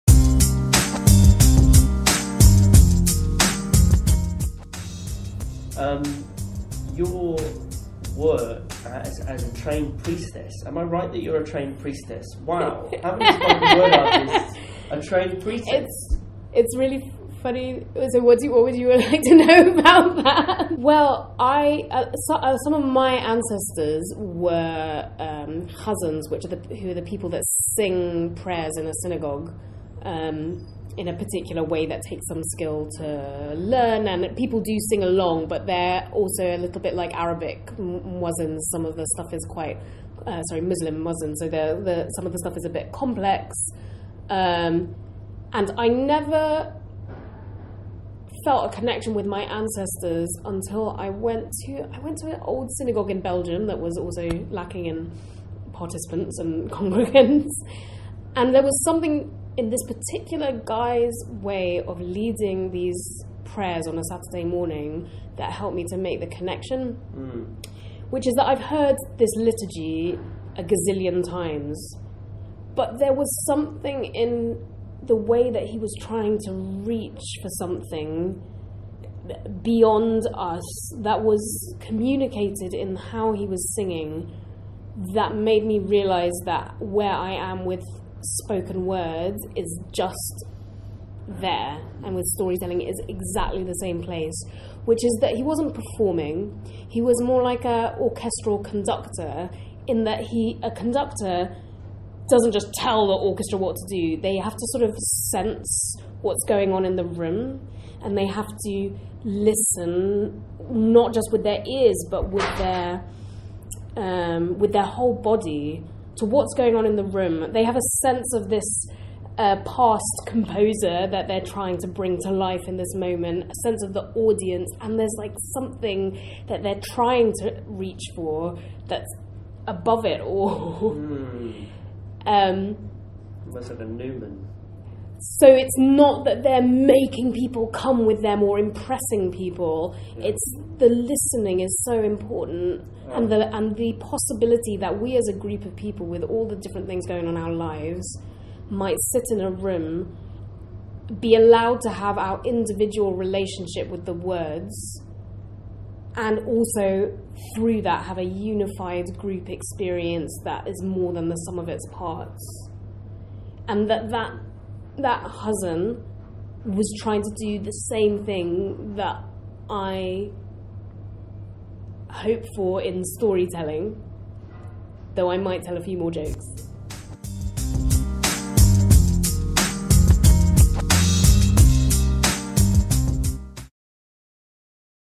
Performing Arts & Religion - Rachel Rose Reid.
Rachel Rose Reid reflects on her craft as a storyteller and a celebrant.

This is part of a longer interview, carried out as research for my book 'Stage Invasion: Poetry & the Spoken Word Renaissance' published in 2019, by Out-Spoken Press. The interview is one of many uploaded onto this playlist, from poets from across the UK.